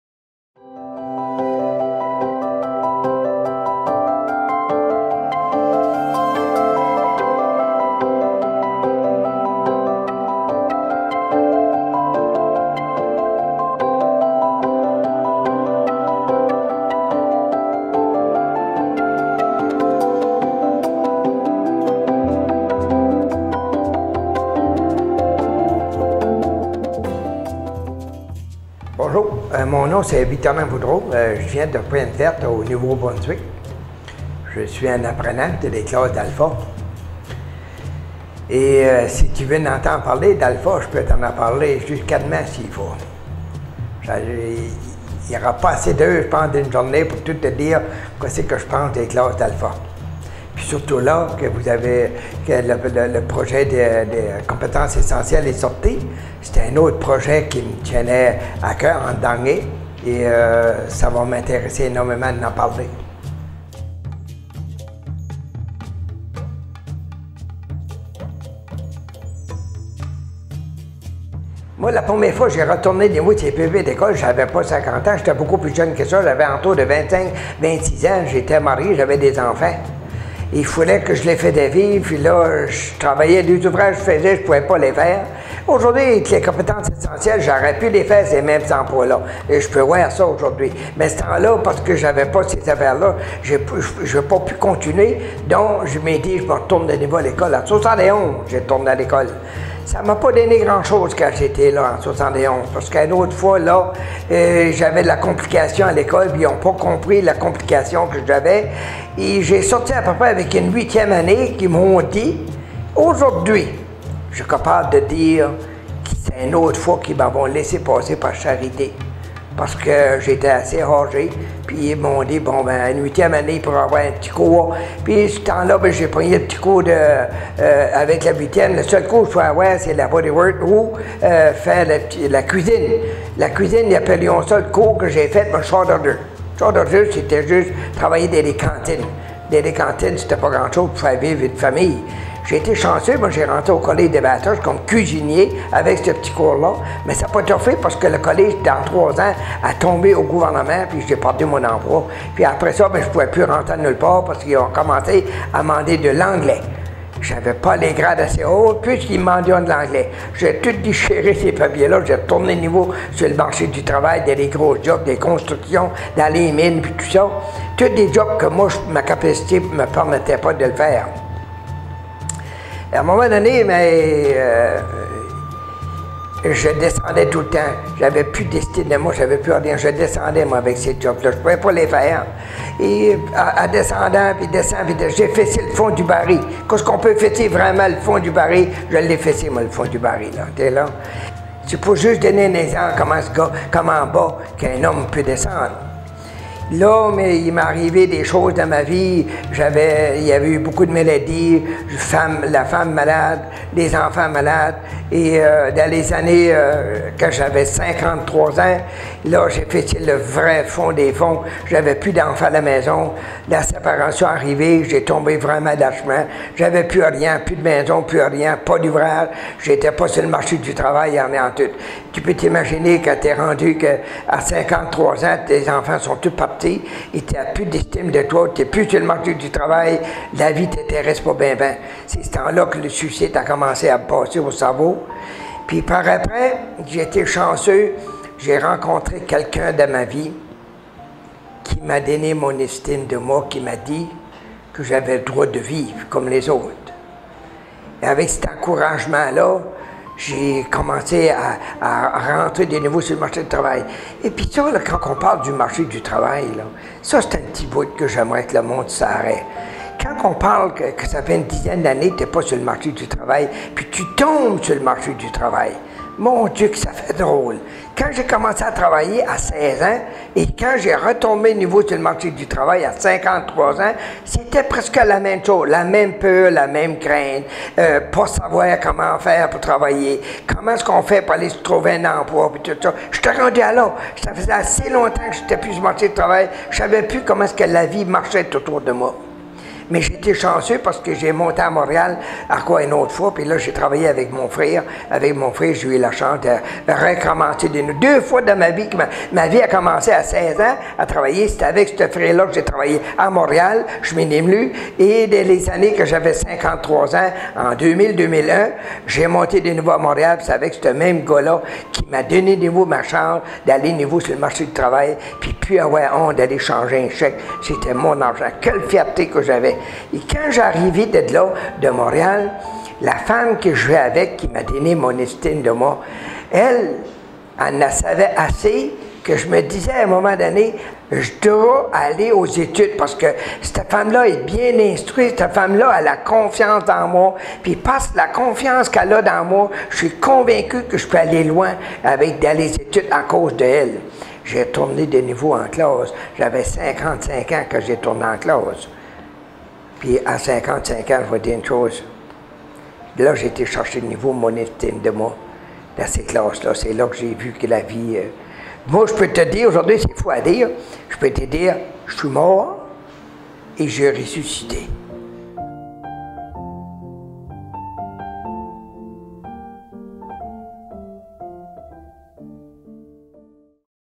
Victorin Boudreau: L'alphabétisme, je peux!
Victorin Boudreau est représentant des personnes apprenantes pour la Fédération d'alphabétisation du Nouveau-Brunswick (FANB), du Réseau pour le développement de l'alphabétisme et des compétences (RESDAC, anciennement FCAF) et du Conseil alpha de Bathurst Chaleur (CACB).

Dans ce segment vidéo, Victorin nous raconte quelques anecdotes mémorables de son parcours personnel, éducatif et professionnel. À travers le récit d'expériences de vie difficiles, il présente les retombées négatives de son analphabétisme, celles-là même qui l'on progressivement conduit à une profonde remise en question personnelle. Une rencontre déterminante le fera plus tard renaître de ses cendres, lui redonnant la confiance et l'estime de lui-même pour reprendre sa vie en main.